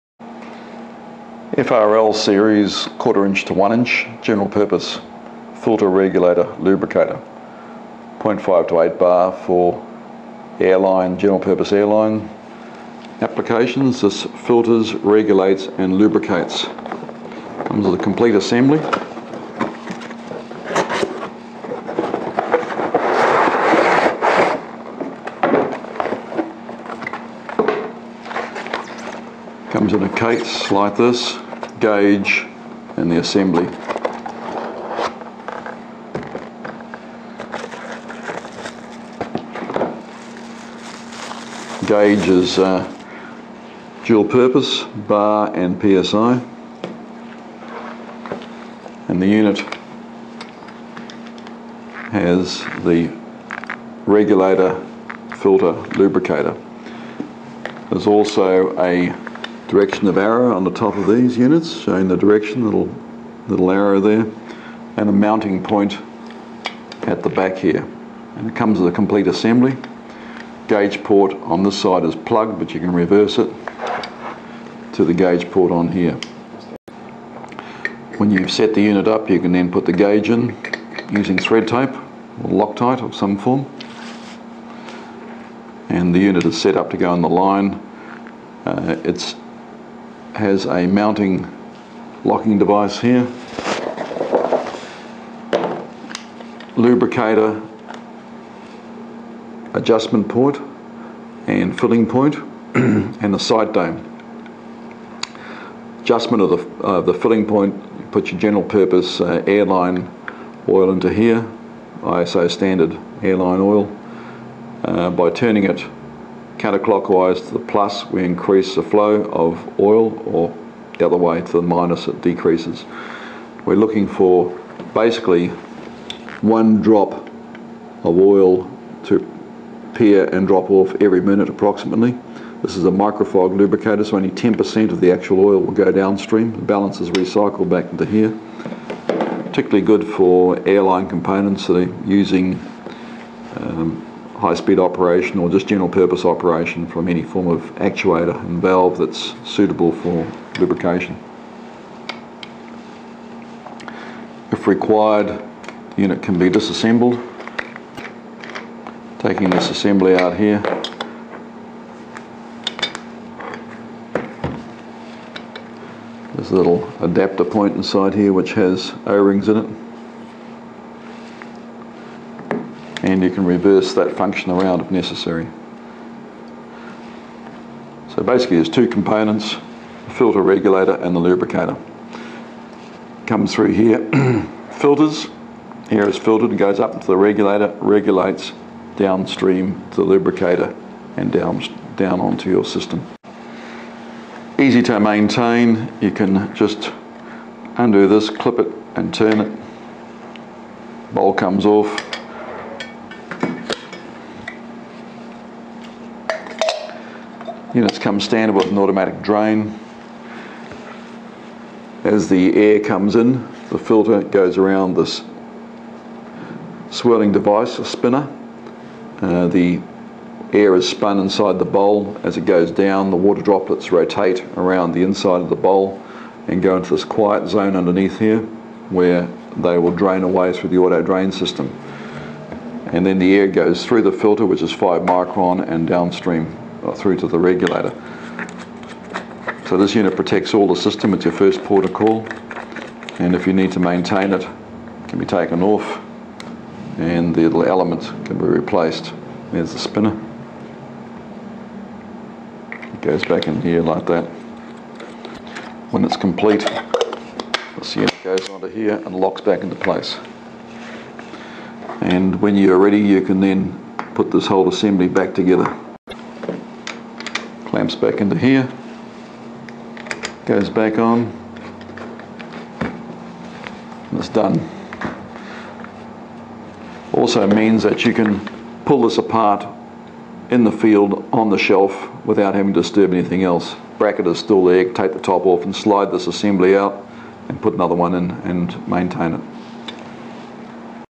Air Line Filter Regulator & Lubricator
Air Line Filter Regulator and Lubricator. Allows the user to regulate air pressure, filter the air and then put a lubrication mist into the clean air.